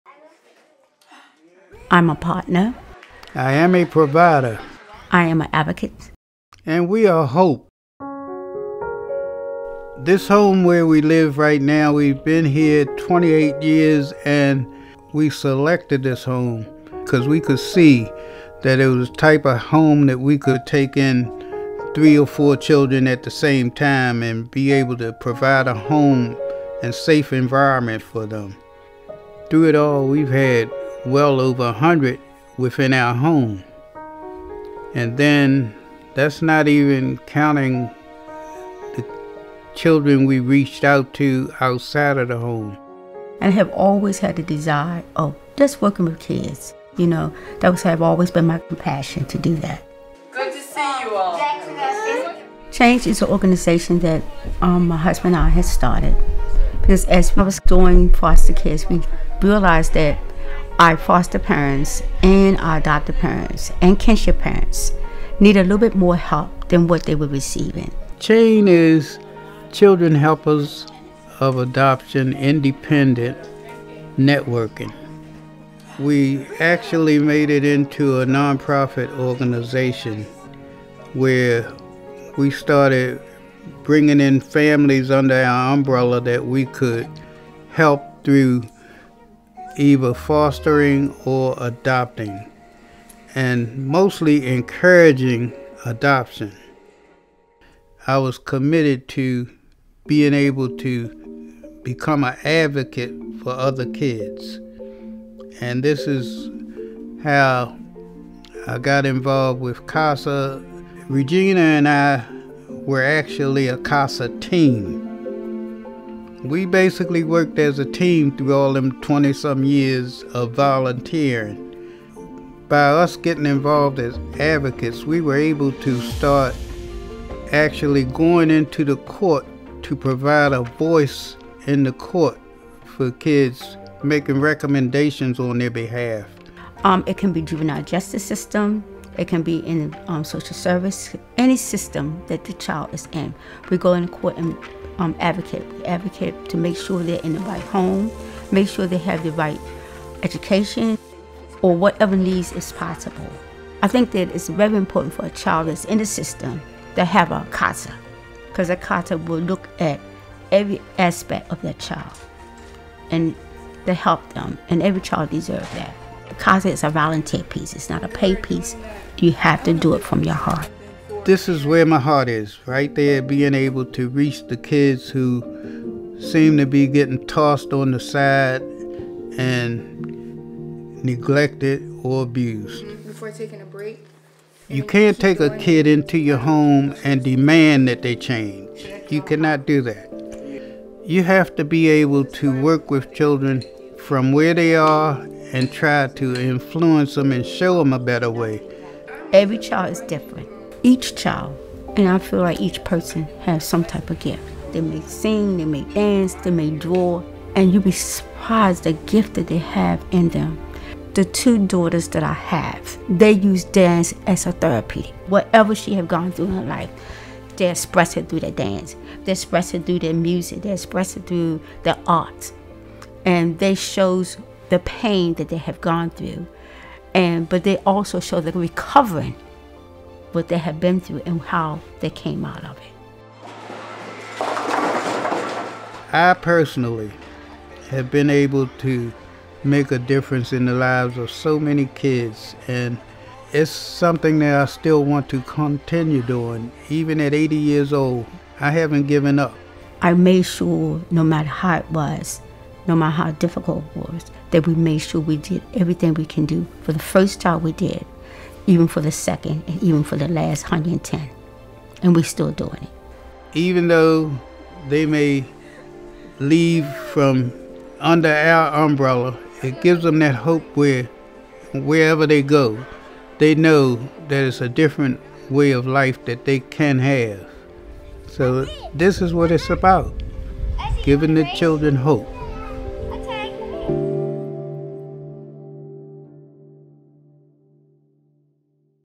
Charles and Regina Harris, 2024 CECA Recipients, Resource Parents
Our 2024 Resource Parents Casey Excellence for Children Awards (CECA) honorees, Charles and Regina Harris, talk about what it takes to raise and mentor children and youth in foster care.